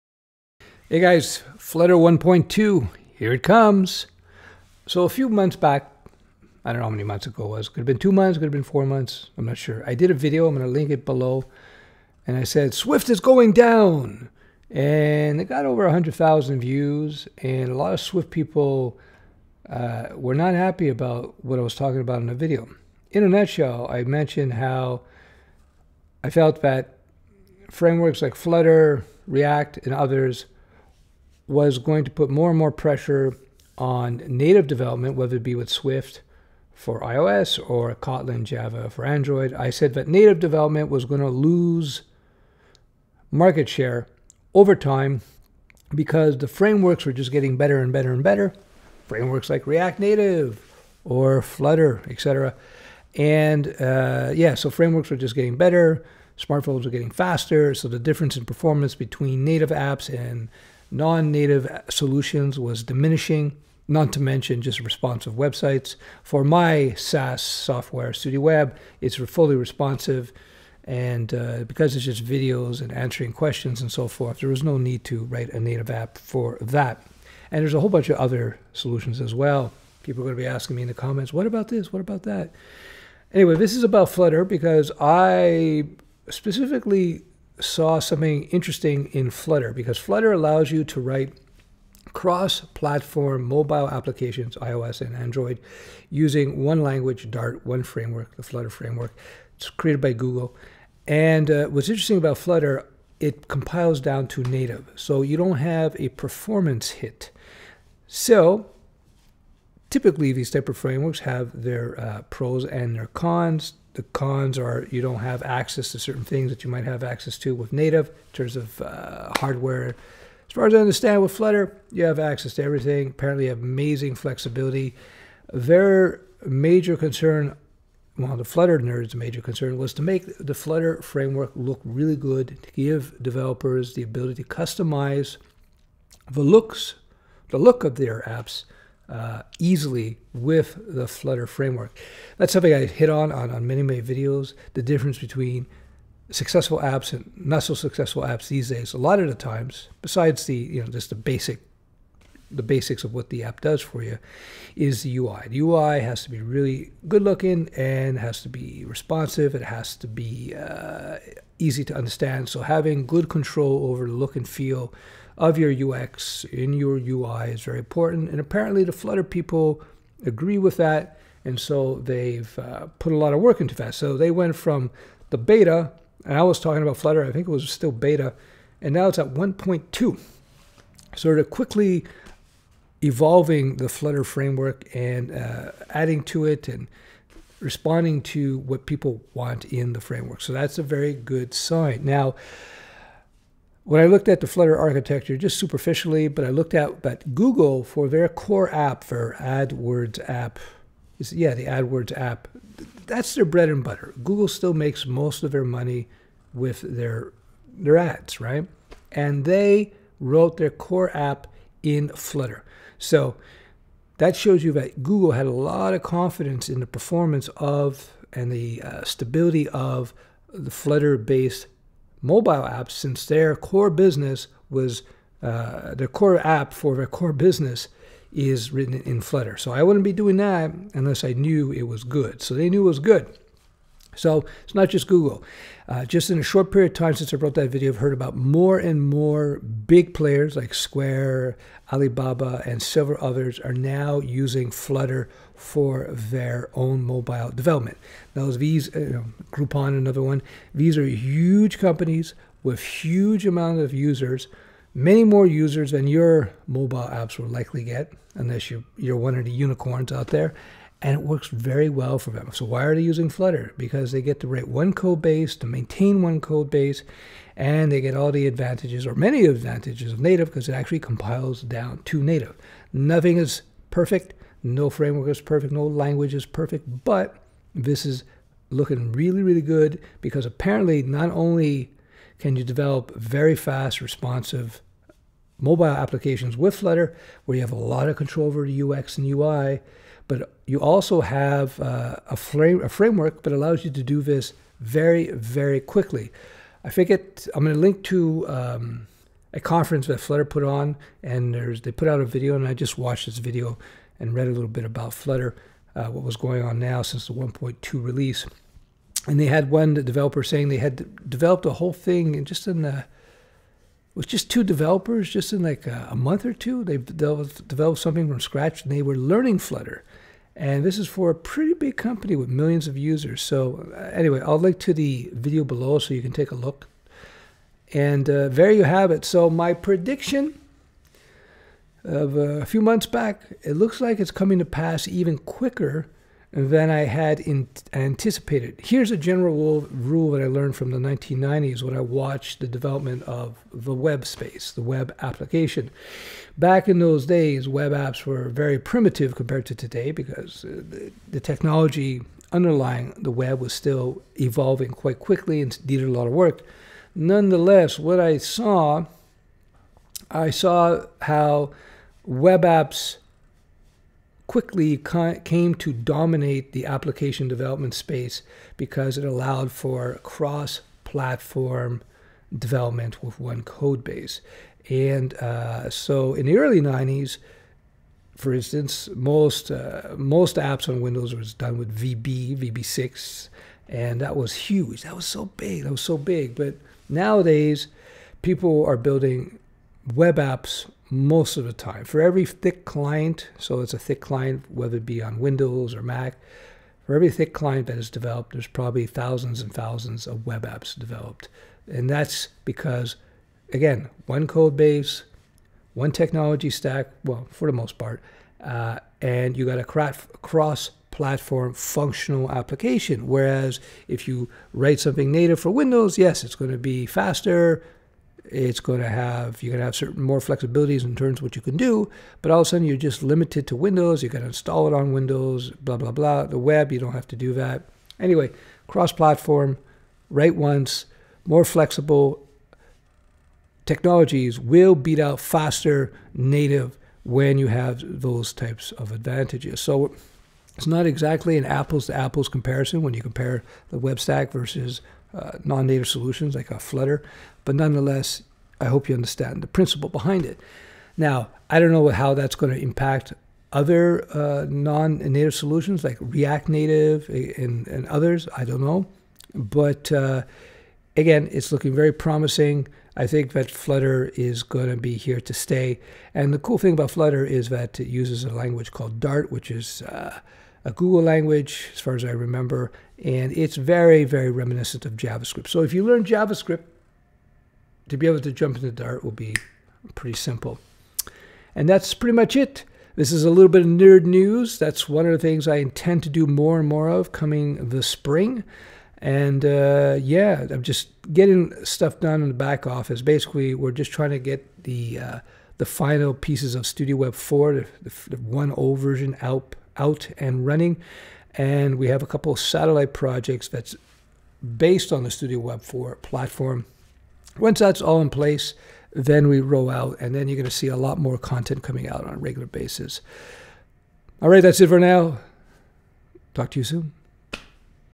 Flutter 1.2 is here and it's HUGE!!!
Flutter 1.2 was just released, and some big companies are using it with great success! Yes my nerd friends, the face of native mobile development is changing indeed ... and fast!

Thanks!

Stef